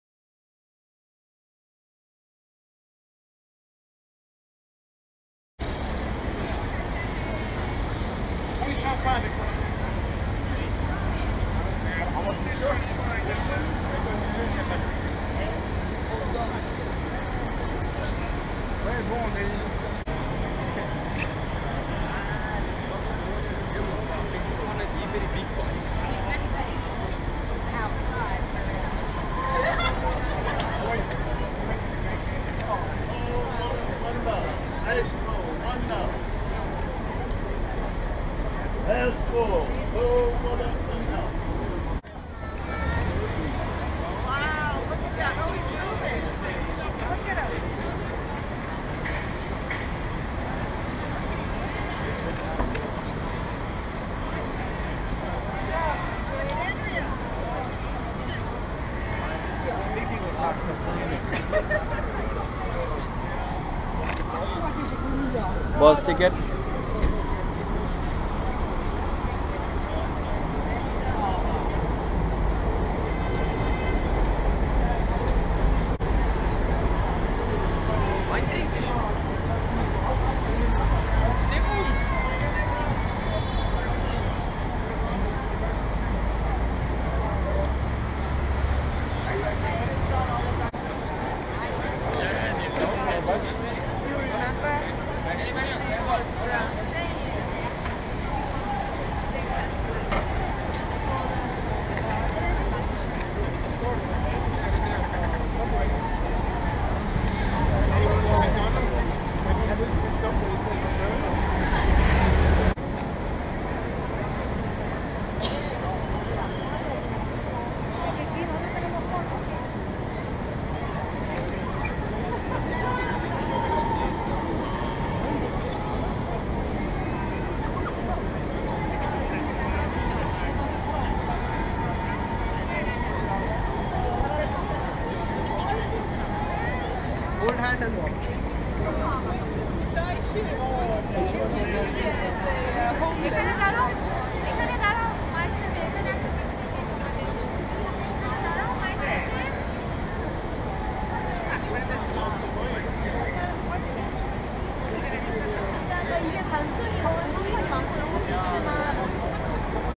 TIME SQUARE | New York City
Take a short stroll through Time Square with me (and my brother)